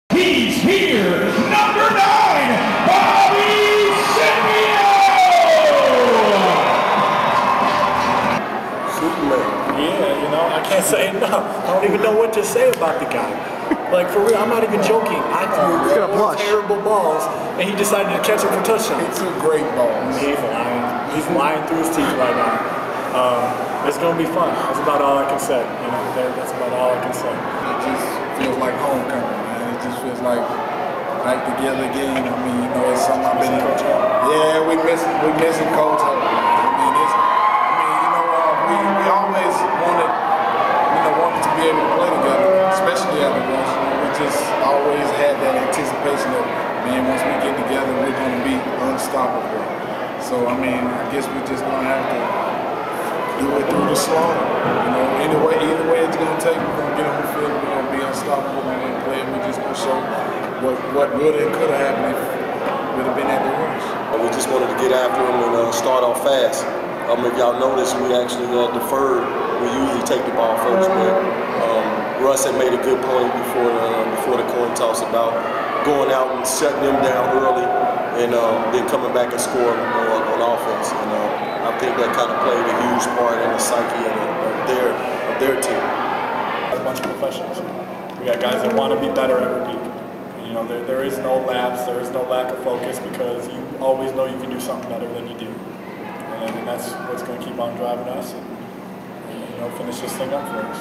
Wisconsin Wolfpack @ Chicago Slaughter 5/16/09
Bobby Sippio scored six touchdowns in his return to indoor football as the Chicago Slaughter defeated the Wisconsin Wolfpack 67-22.  Sippio, Russ Michna, and DeJuan Alfonzo discuss the game.